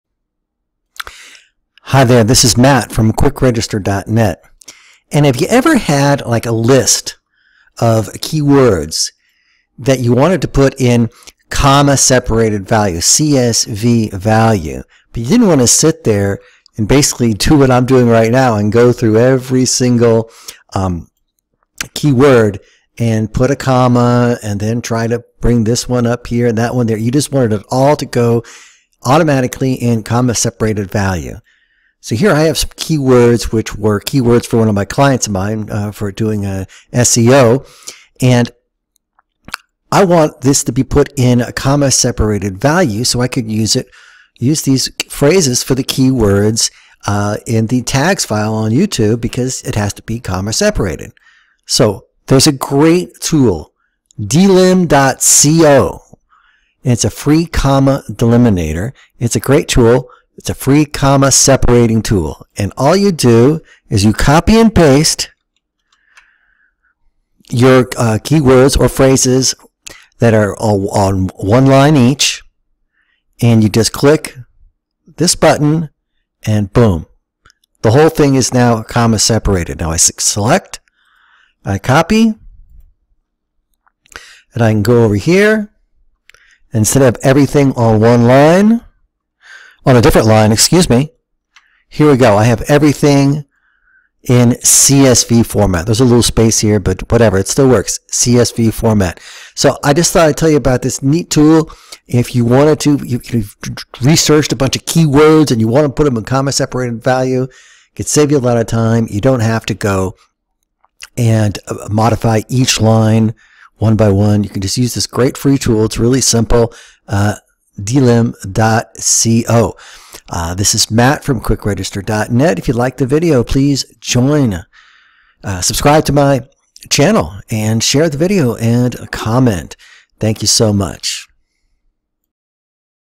Free Comma Separating Tool (How to covert data to a comma-delimited list)
Do you often need to take a spreadsheet of data and convert to a comma-delimited list?

Be it for taking a list of zip codes or names to make an SQL query, or to take data from a CSV and be able to paste into an array. At delim.co we make that just a little easier.

Enter your non-delimited data on the left, hit the button, and boom, separated data on the right. Special configs are below if the defaults aren't what you need!